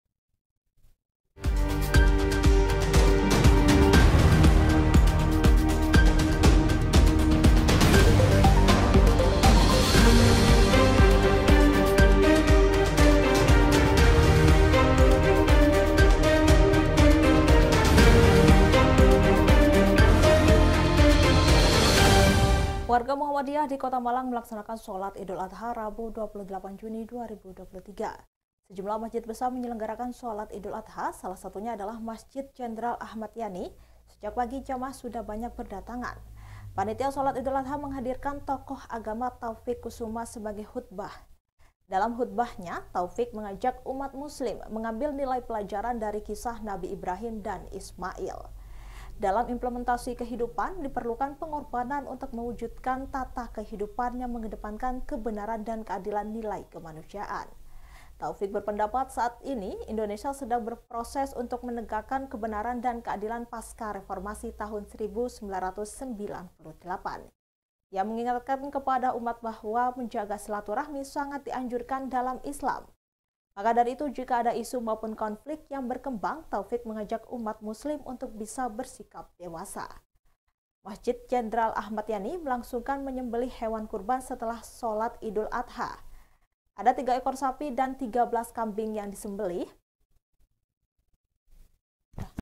Warga Muhammadiyah di Kota Malang melaksanakan salat Idul Adha di Masjid Jenderal Ahmad Yani
VP : rizal

TRIBUNJATIMTIMUR.COM - Warga Muhammadiyah di Kota Malang melaksanakan salat Idul Adha di Masjid Jenderal Ahmad Yani